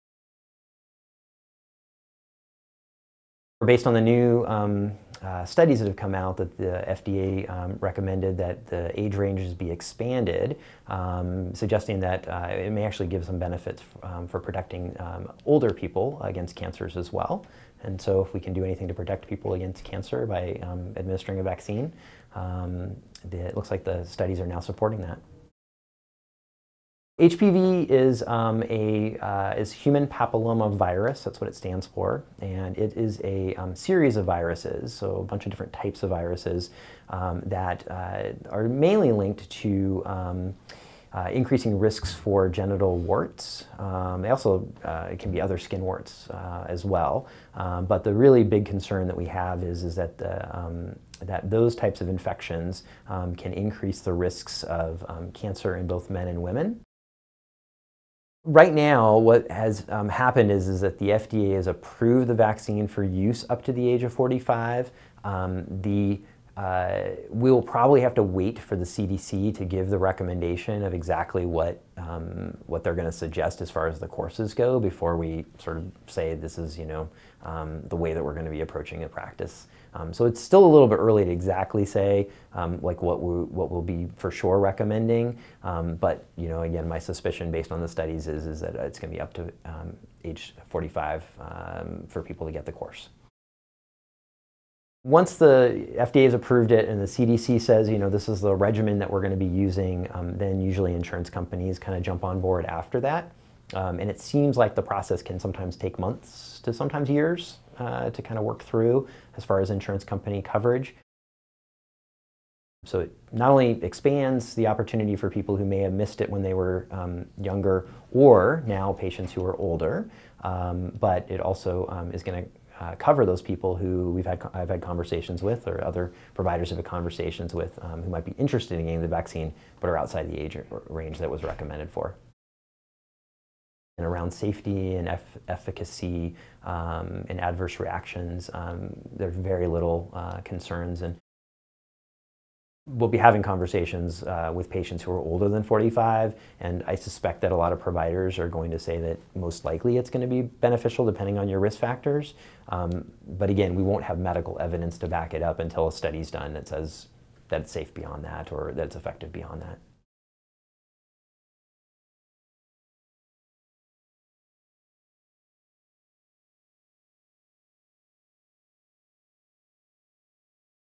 FDA OKs HPV vaccine to age 45
The FDA approved making Gardasil 9, the vaccine to protect against human papillomavirus, available to women and men up to age 45. The vaccine had initially been rolled out to individuals ages 9 to 26. Matthew Jaffy, associate medical director of the UW Neighborhood Clinics, discusses the studies supporting the change and what the vaccine protects against.